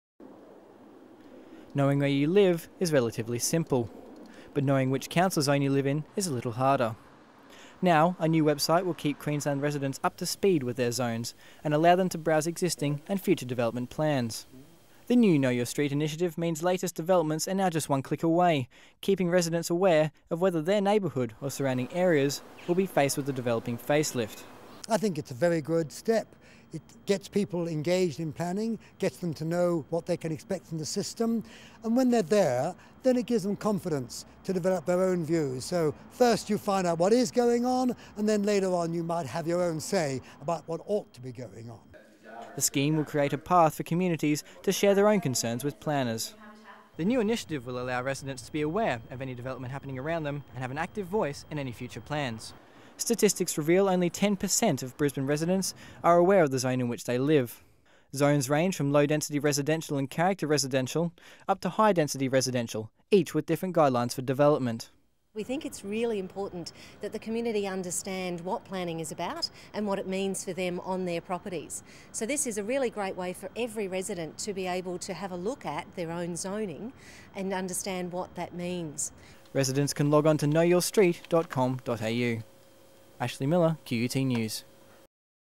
How to know your street
Knowing your street has never been easier, with the Planning Institute of Australia launching a new interactive website.

The site details council's current and future development plans, made readily available to the public.

Ashley Miller reports.